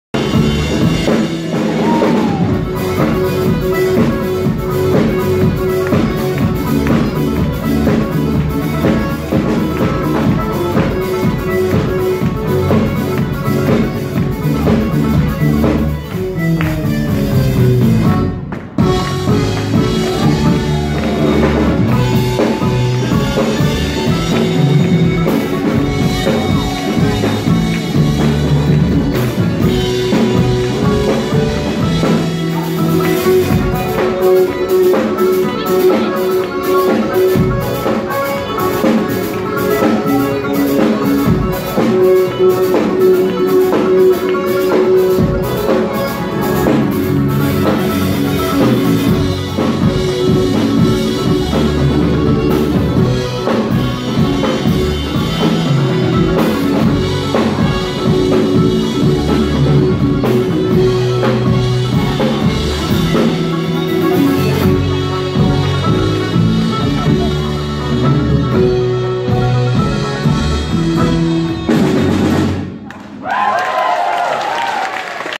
Cúig - Irish Festival of Oulu 2022
Cúig is a five-piece band from the North of Ireland tearing up the rule book with their bold approach to their native musical tradition.
With self penned songs and a fiery collection of tunes, Cúig have fast made an international name for themselves with dynamic and powerful performances. Their shows have left audiences on their feet on both sides of the Atlantic with extensive tours in the US, Europe and Ireland. Now, they land in Oulu for the first time.

date and location: October 15.2022 Valve Concert Hall, Culture Centre, Irish Festival of Oulu, Finland, Oulu

Miceál Mullen - banjo, mandolin
Rónán Stewart - fiddle, uilleann pipes, vocals
Cathal Murphy - percussion, guitar, vocals
Eoin Murphy - button accordion
Ruairí Stewart – guitar